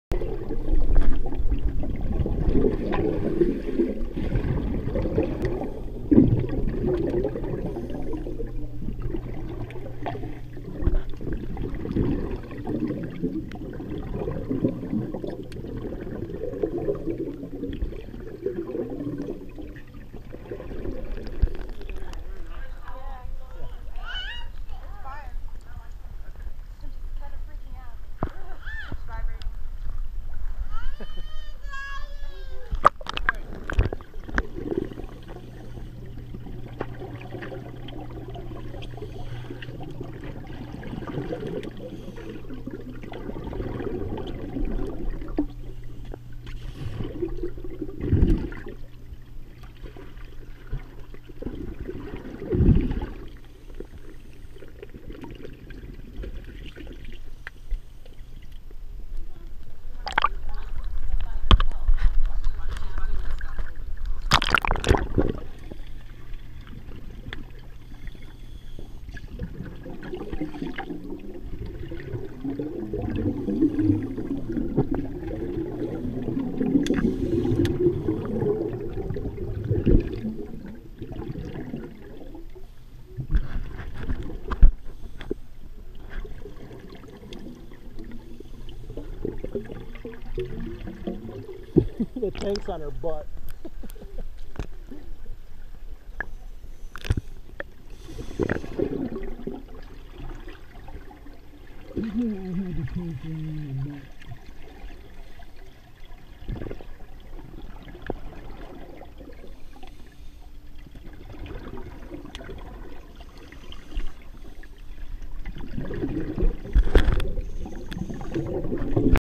pool01 Broadband
Fun in the pool after a Memorial Day picnic with family and friends.